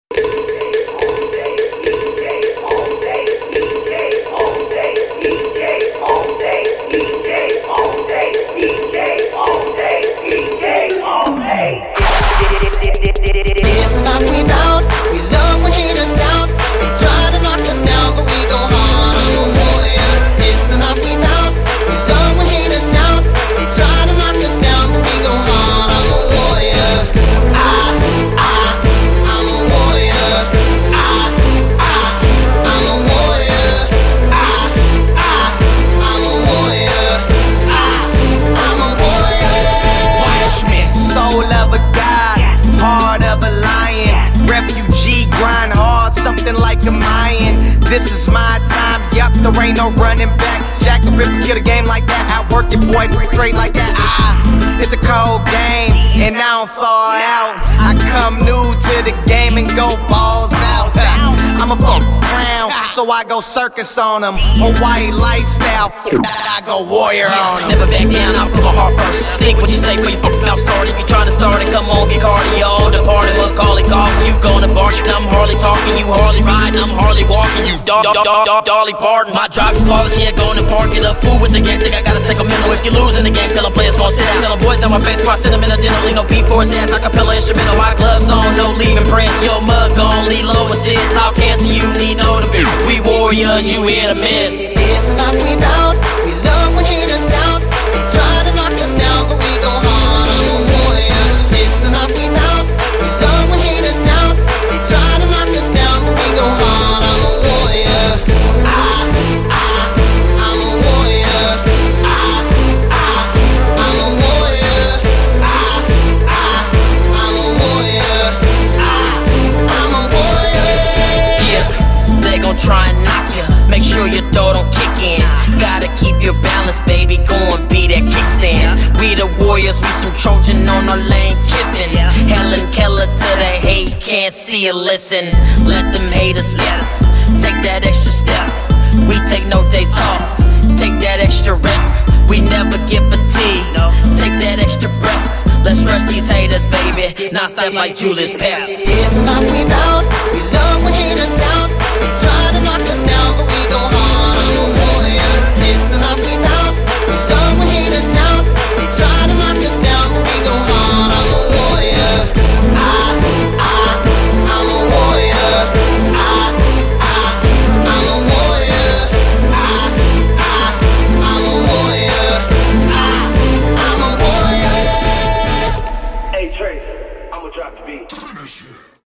David Hafoka University of Hawaii NFL Pro Day
David Hafoka, #96 Defensive Tackle University of Hawaii NFL Pro Day Workout, Carson, CA March 5, 2012.